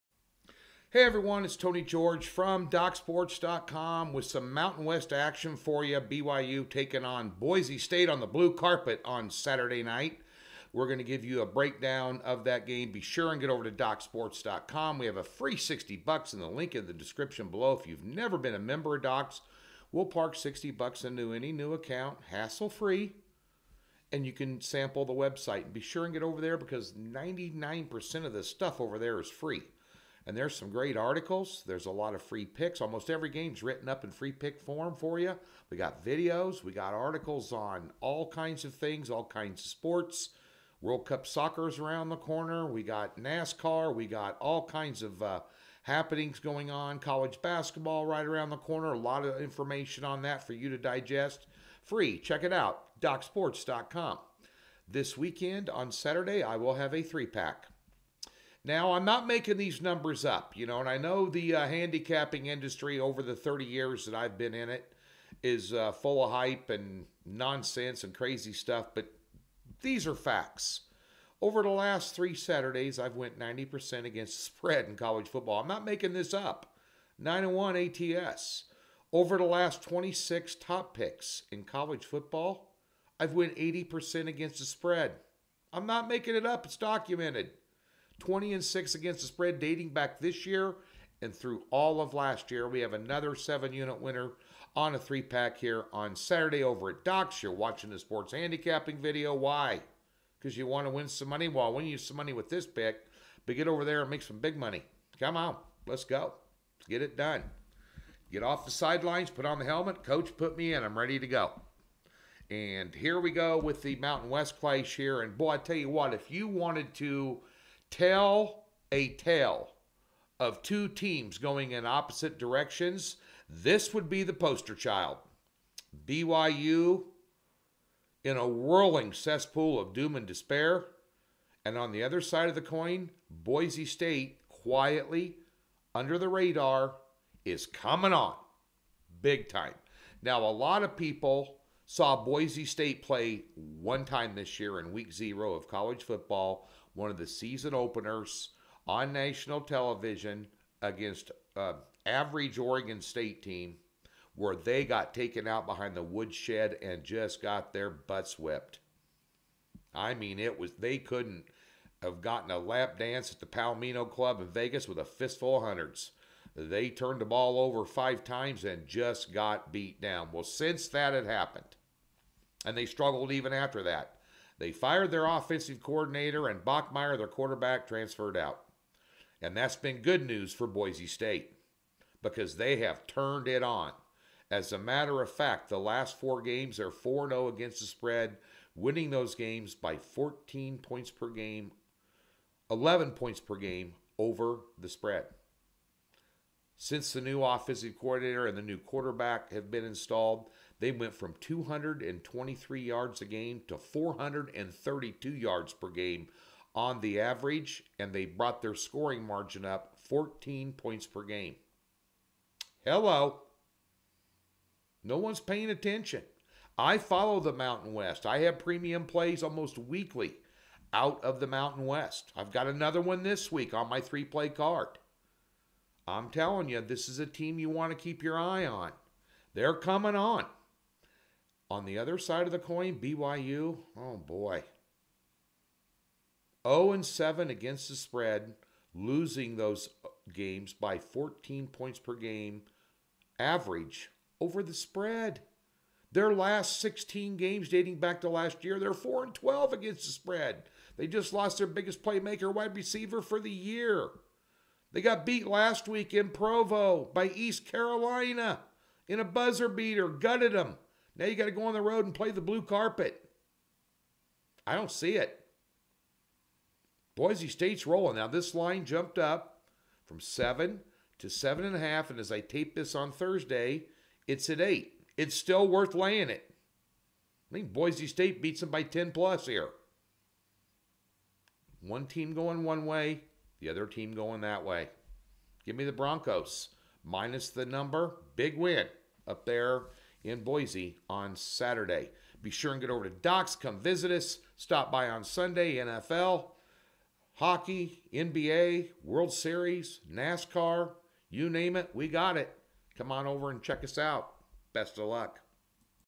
Free Football Pick BYU Cougars vs Boise State Broncos Prediction, 11/5/2022 College Football
Boise State Broncos vs BYU Cougars College Football Pick and Prediction 11/5/2022. The BYU Cougars travel to Boise, ID to face the Boise State Broncos at 7:00PM EST at Albertsons Stadium.

Doc's Sports provides College Football picks against the Spread, expert predictions, betting tips and college football odds on this matchup of BYU Cougars vs Boise State Broncos.